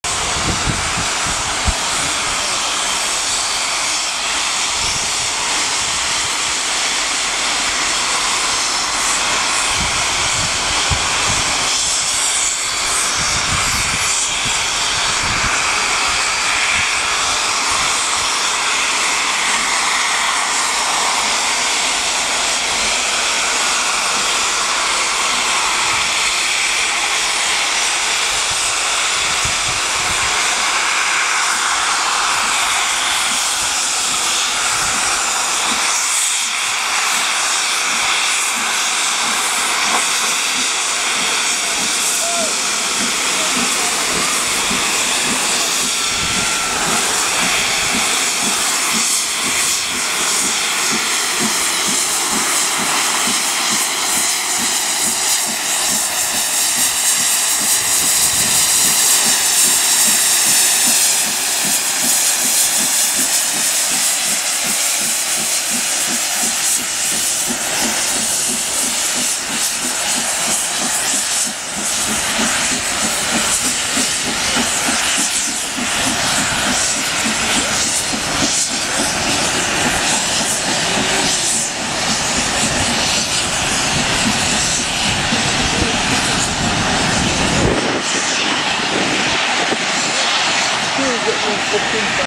46233 "Duchess of Sutherland" Leaving York
duchess of sutherland leaving york on the way to Tyseley after coming back from scarborough